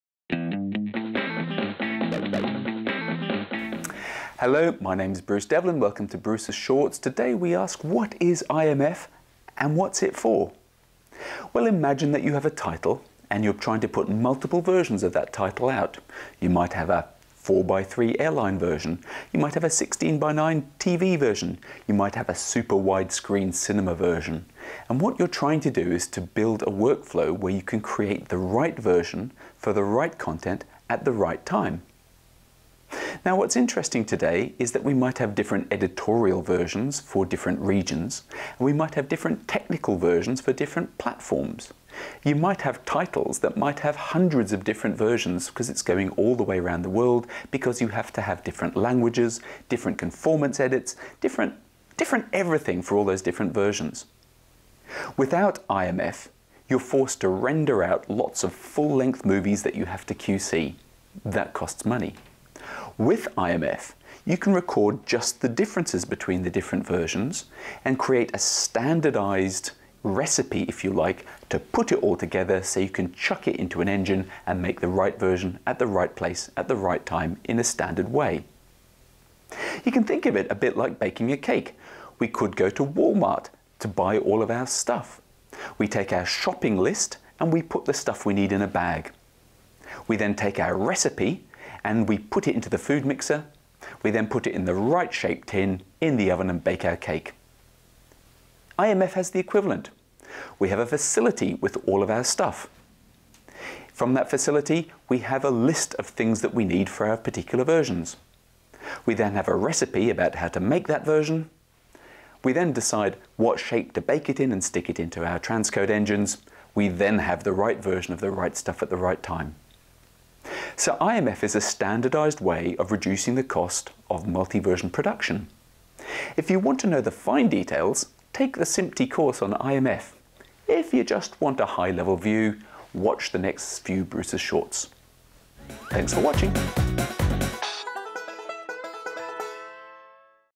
bs401 Why does IMF exist? (original version)
IMF is a new standard from SMPTE - ST 2067. It is aimed at improving the automation for multi-version production. This episode explains the IMF basics.